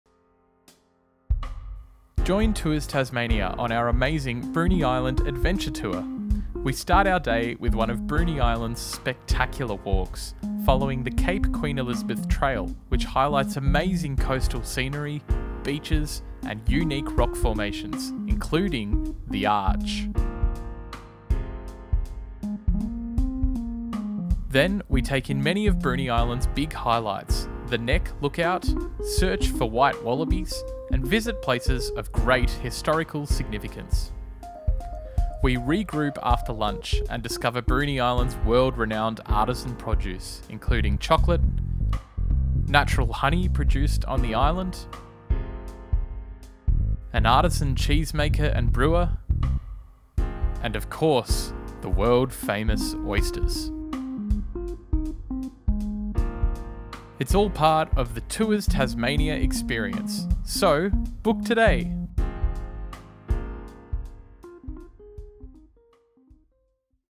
Tours Tasmania Bruny Island Adventure
If you like spectacular scenery and gourmet local produce, Bruny is your place; an island off an island off an island at the bottom of the work.  ​ @tourstasmania8956